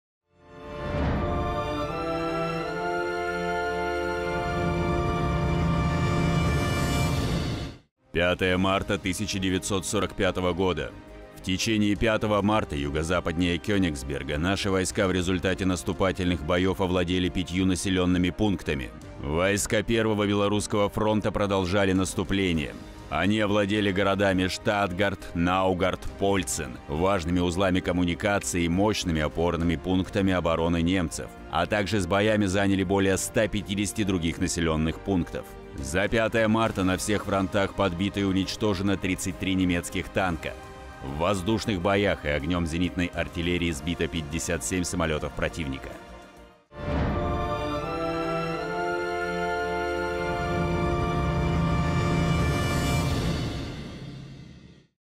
Военные хроники. 5 марта 1945 года. До Победы - 66 дней
В течение 5 марта юго-западнее Кенигсберга наши войска в результате наступательных боёв овладели 5 населенными пунктами.
Войска 1-го белорусского фронта, продолжали наступление. Они овладели городами Штаргард, Наугард, Польцин  - важными узлами коммуникаций и мощными опорными пунктами обороны немцев, а также с боями заняли более 150 других населённых пунктов.
За 5 марта на всех фронтах подбито и уничтожено 33 немецких танка. В воздушных боях и огнём зенитной артиллерии сбито 57 самолётов противника.